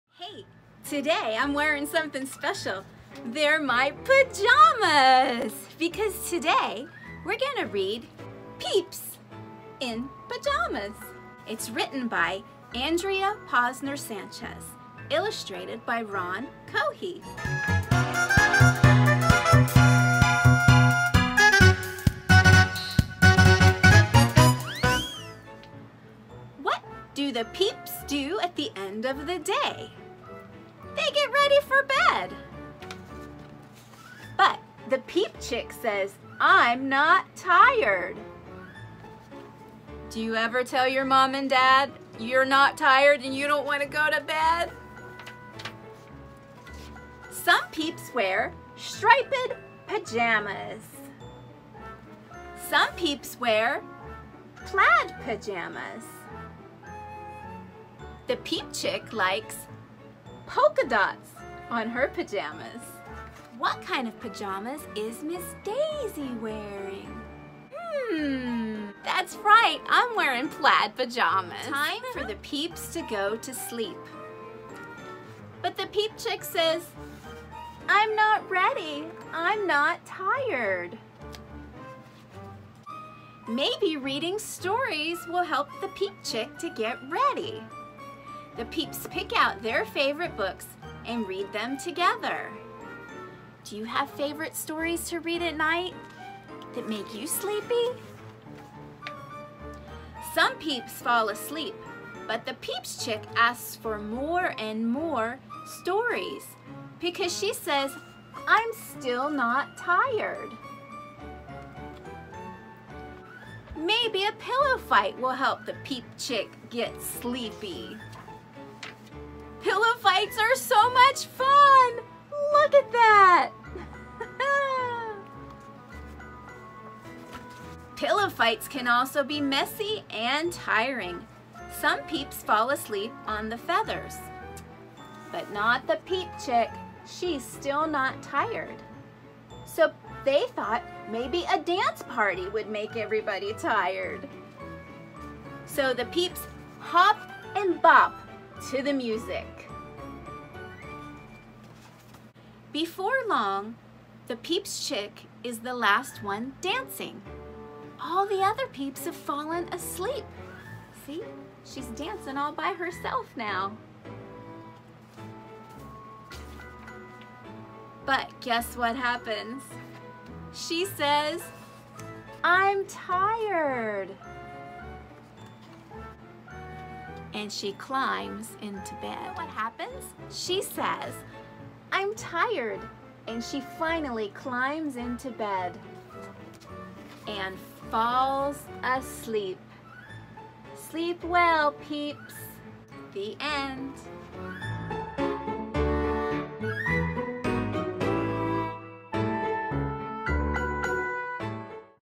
Peeps in Pajamas| Storytime with Miz Daisy
It’s bedtime and peep chick is not tired! Has that ever happened to you? Let’s see how peep chick gets to sleep.